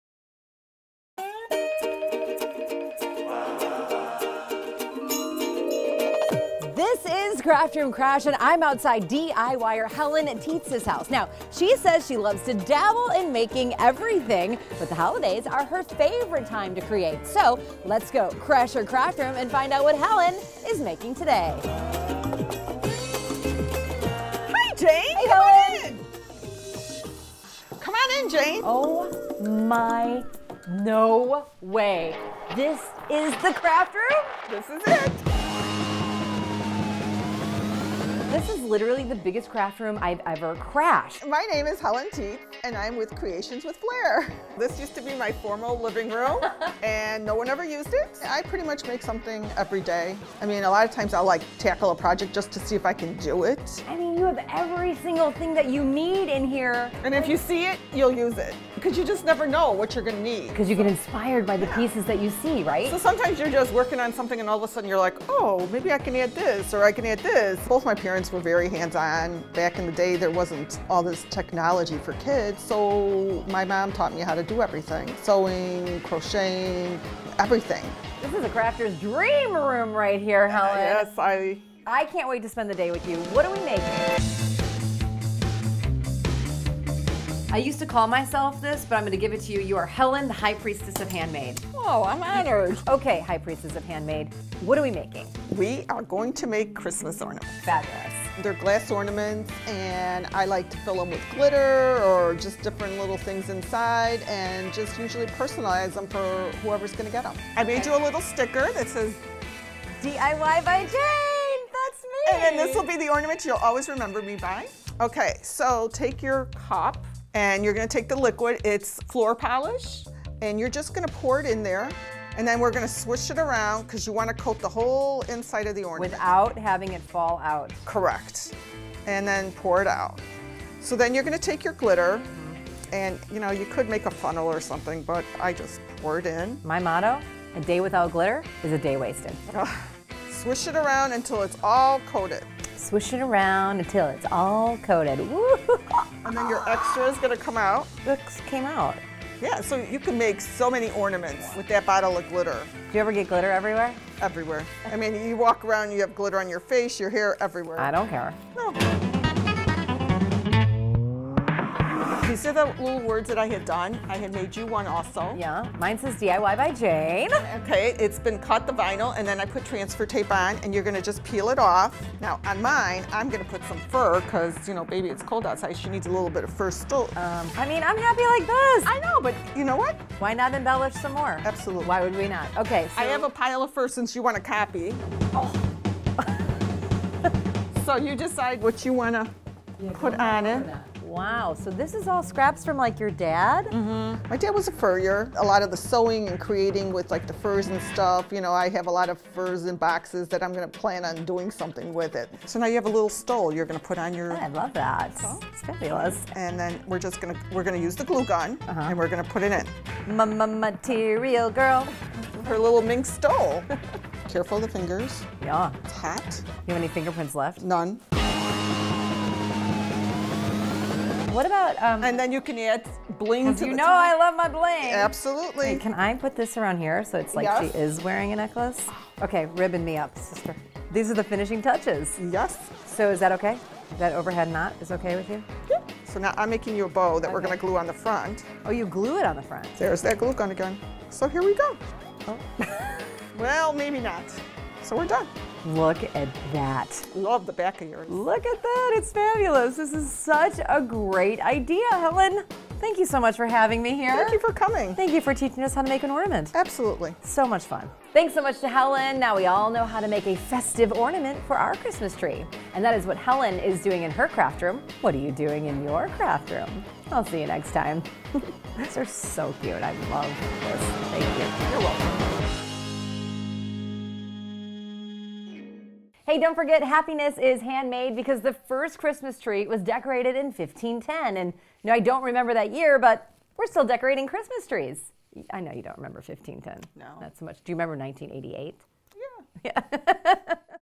Craft Room Crash: Glitter Glass Ornaments
In this episode of Crash Room Crash, Jane checks in with DIYer Helen Tietz to learn her tricks on how to make ornaments with love and a little floor polish.